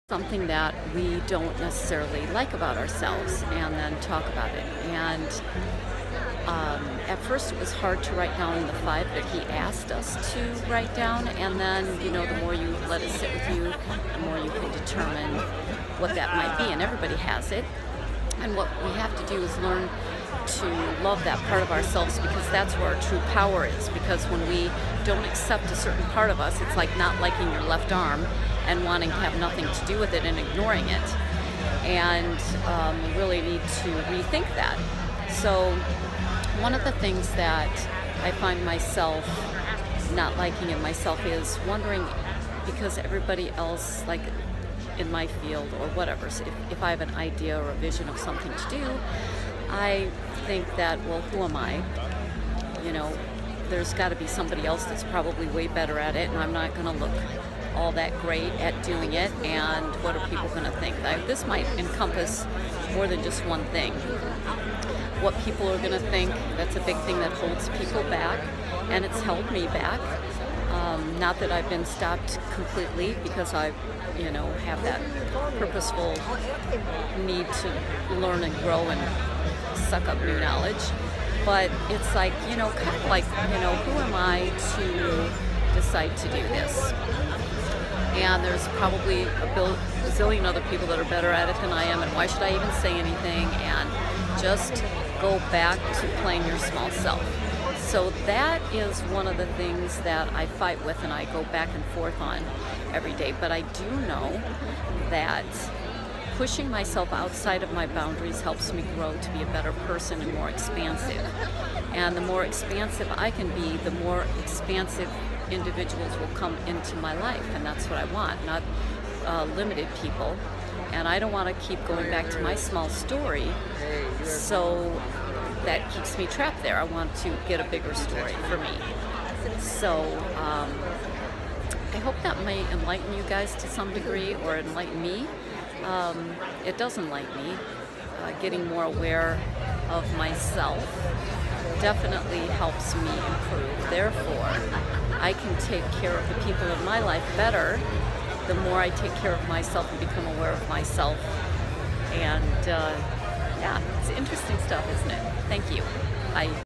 What are your fears?
One of the assignments we had at evolving out loud with Kyle Cease was to list five things that we didn’t like about ourselves. Most of these are some type of fear that we have that hold us back in life and keep us in our small story. It’s time to become self-aware and identify those so you can overcome them. Get out and play big!